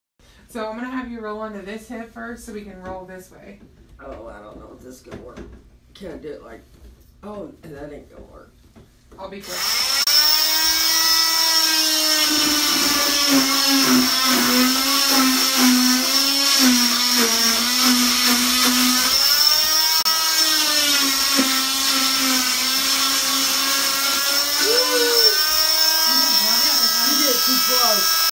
Cast Removal Has Mom Concerned || ViralHog
Occurred on February 11, 2019 / Farmington, Missouri, USA

"My mom had her cast removed."